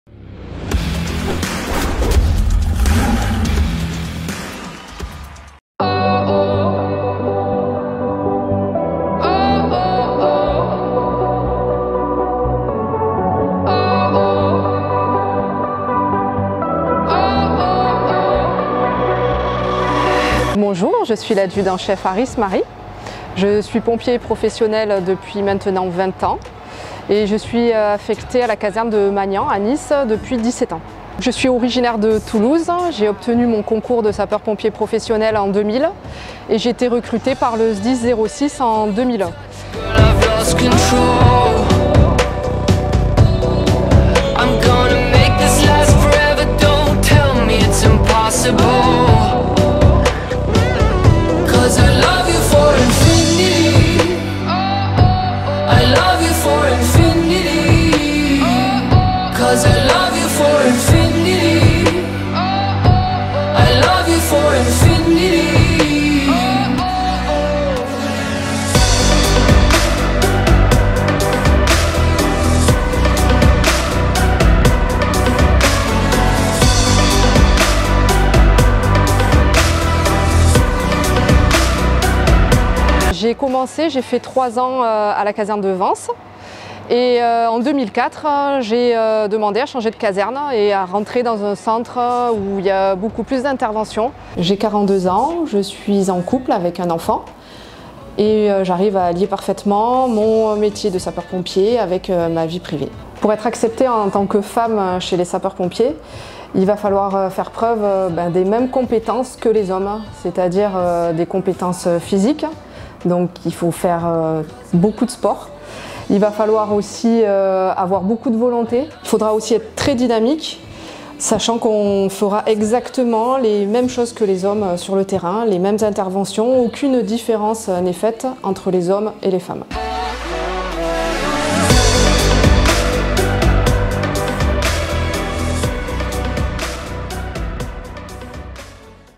Portrait #2 : A/c Marie ARRIS (Pompiers de Nice)
portrait #2 | Rencontre avec l’A/C Marie ARRIS, femme Sapeur-Pompier professionnelle de Nice affectée au Cis Magnan (Section 3). 🎥
L’occasion de saluer toutes les femmes engagées comme Sapeurs-Pompiers de France 🇫🇷
🆔 Compagnie de Nice

- Concept Portrait : Nous vous présentons un Sapeur-Pompier de la Compagnie de Nice.
- Réalisation : SGC M.STARK
- Sous Couvert du Lt Colonel Olivier PAULETTI, Commandant de la Compagnie de Nice.